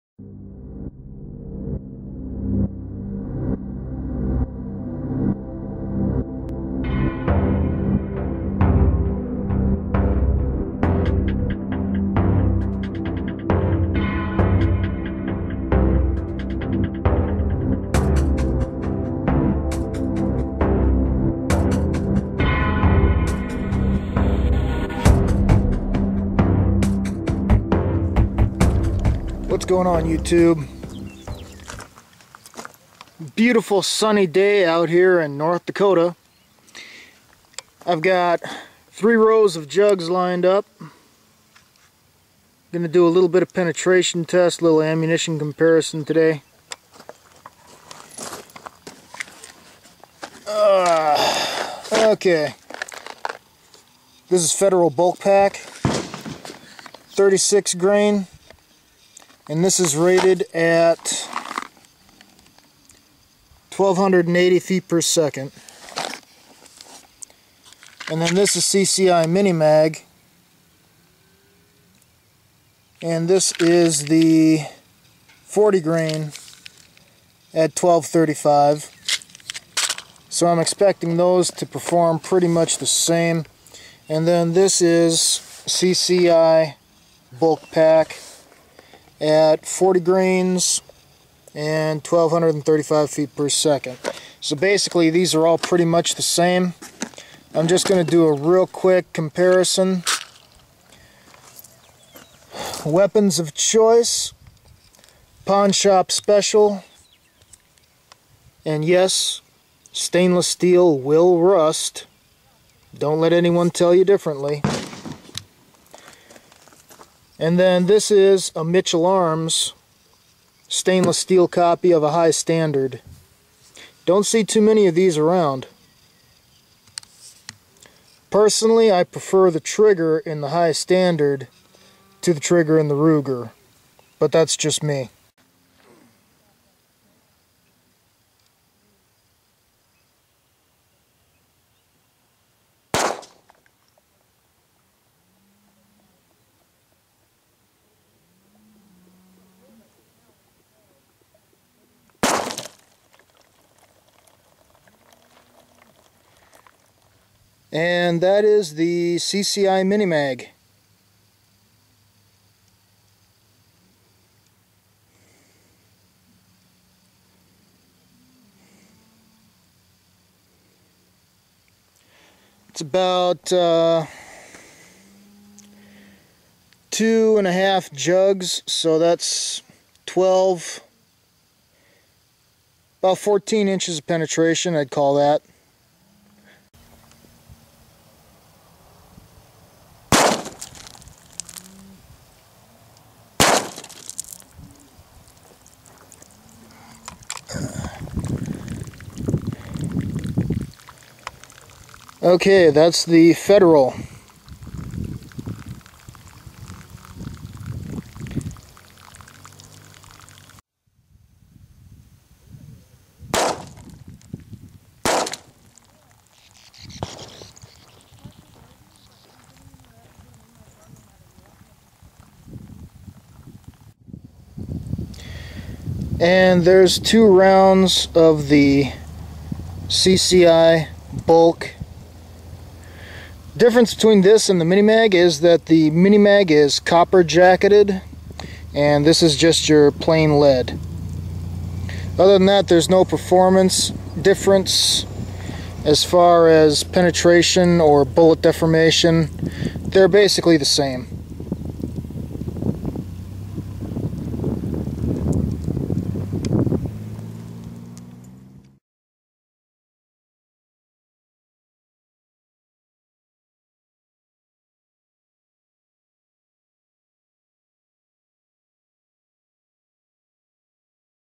22lr Penetration Test V1
I test the penetration of 3 common bulk ammo from 2 different pistols with 5" barrels.

Regardless of ammo or pistol the performance was the same: From a 5" barrel all loads penetrated roughly 14" to 16" of water at a distance of 30 feet.